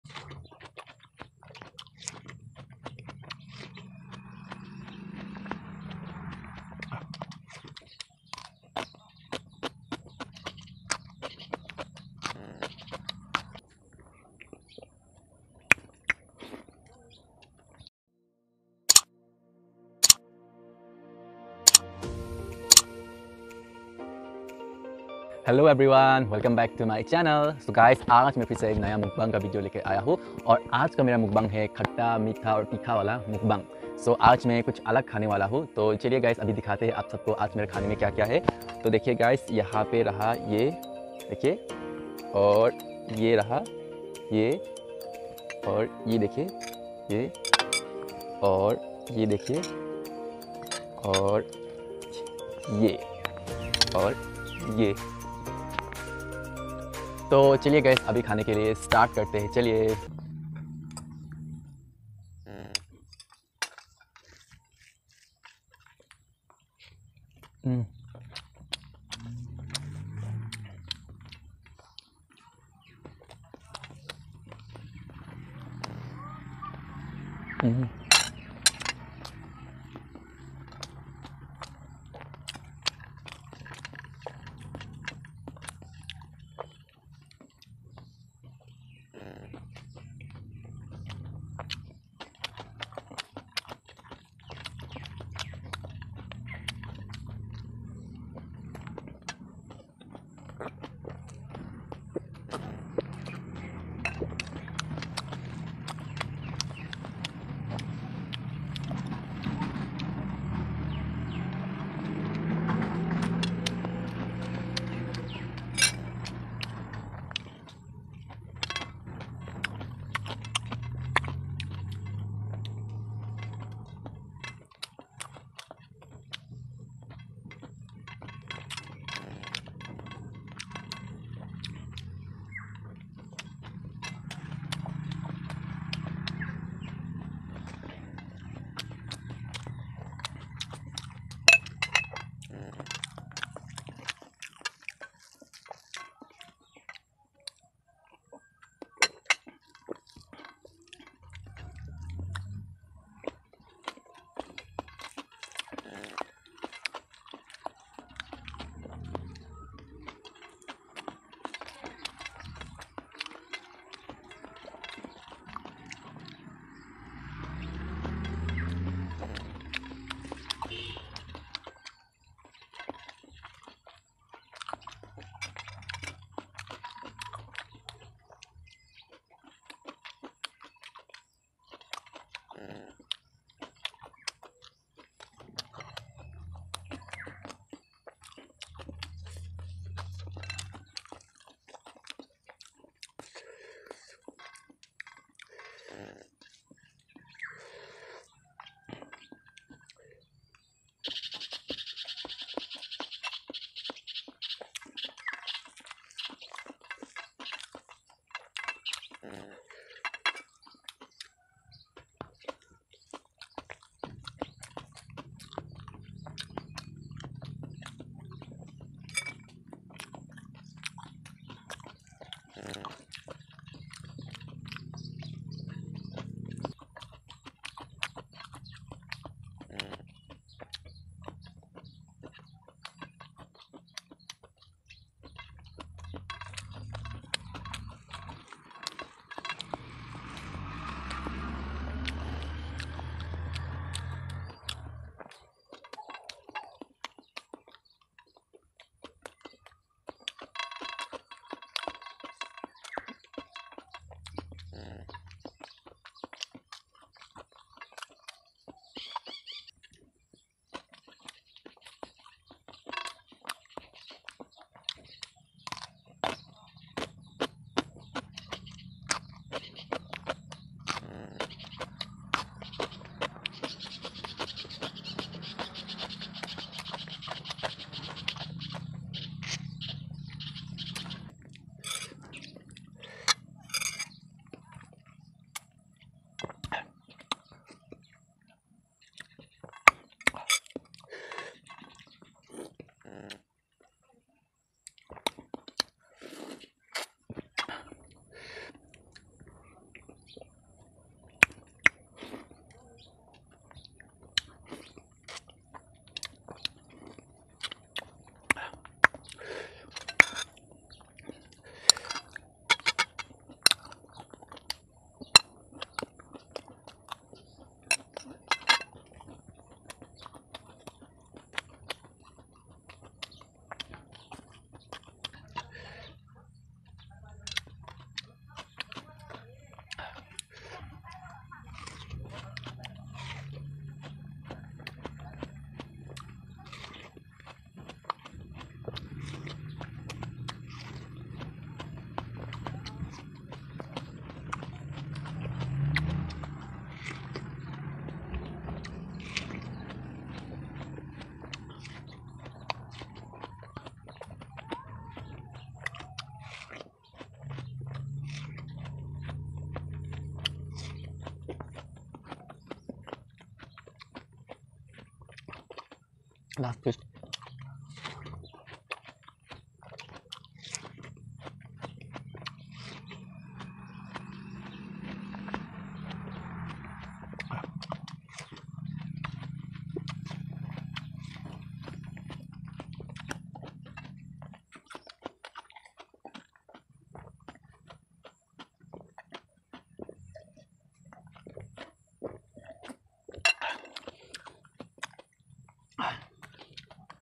Tantalizing Mukbang Experience: Indulging in Unique Khatta, Meetha, and Teekha Foods
Unique Food Mukbang//Eating Khatta, Meetha And Teekha Food Mukbang 😋